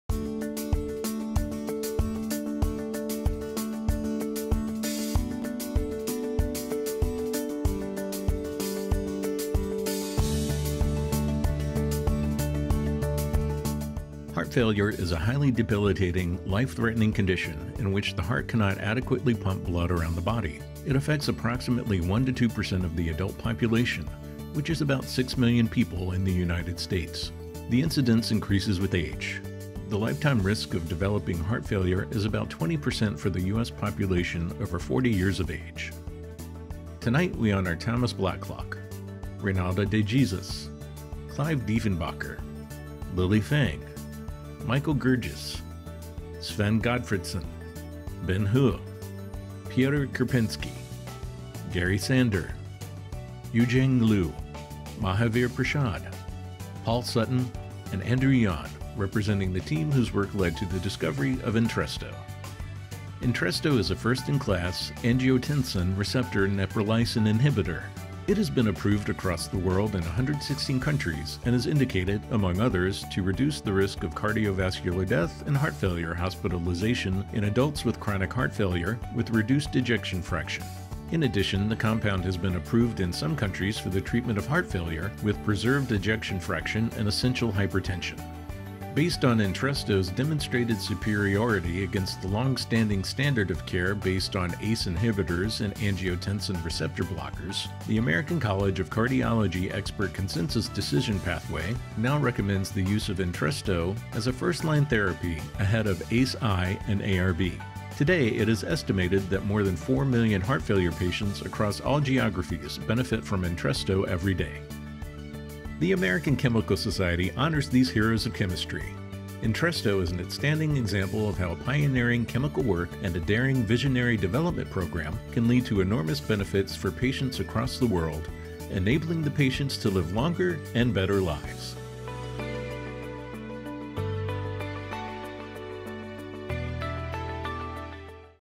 Novartis – 2021 Heroes of Chemistry
The American Chemical Society honors these Heroes of Chemistry from Novartis whose work led to the development of ENTRESTO® (sacubitril/valsartan). ENTRESTO® (sacubitril/valsartan) is an angiotensin receptor neprilysin inhibitor (ARNI) delivering concomitant neprilysin inhibition and blockage of the angiotensin II type-1 receptor, approved to reduce the risk of cardiovascular death and heart failure (HF) hospitalization in adults with chronic HF.